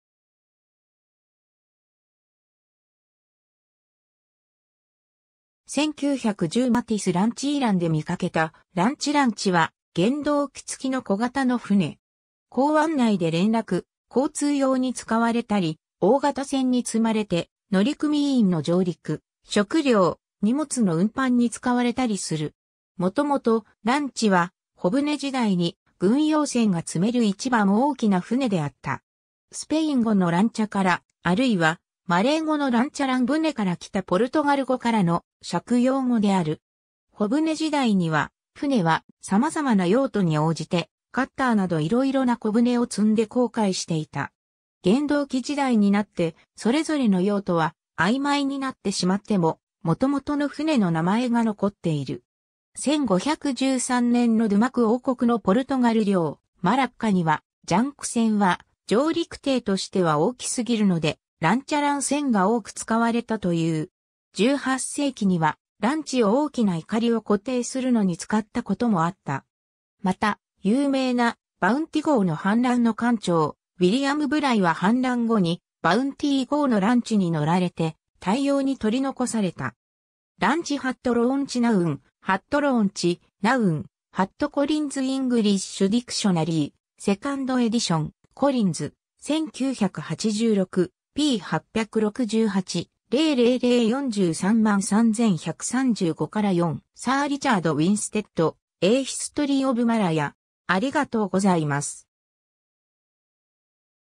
ランチ (船)
1910Mathisランチ（ニューヨーク州サラナック湖で） イランで見かけたランチ（漁業用） ランチ（英語: Launch）は原動機付の小型の船。
港湾内で連絡、交通用に使われたり、大型船に積まれて乗組員の上陸、食料・荷物の運搬に使われたりする。
 もともと、ランチは帆船時代に、軍用船が積める一番大きな船であった。
スペイン語のランチャ（lancha、「艀」の意味）から、あるいは、マレー語のランチャラン船（lancharan、lanchar「容易に速度を出せる」から）からきたポルトガル語からの借用語である。
 帆船時代には、船は様々な用途に応じて、カッターなどいろいろな小船を積んで航海していた。
原動機時代になって、それぞれの用途はあいまいになってしまっても、もともとの船の名前が残っている。
 1513年のドゥマク王国のポルトガル領マラッカには、ジャンク船は上陸艇としては大きすぎるので、ランチャラン船が多く使われたという。
 18世紀には、ランチを大きな錨を固定するのに使ったこともあった。
また、有名な「バウンティ号の反乱」の艦長・ウィリアム・ブライは反乱後にバウンティー号のランチに載られて、大洋に取り残された。
 ^ ランチ（コトバンク） ^ Launch (3) noun (Merriam Webster) ^ Launch, noun (2) (Oxford Dictionaries) ^ Collins English Dictionary, Second Edition, Collins (London & Glasgow), 1986, p. 868, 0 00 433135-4. ^ Sir Richard Winstedt,"A History of Malaya" (Marican, 1962)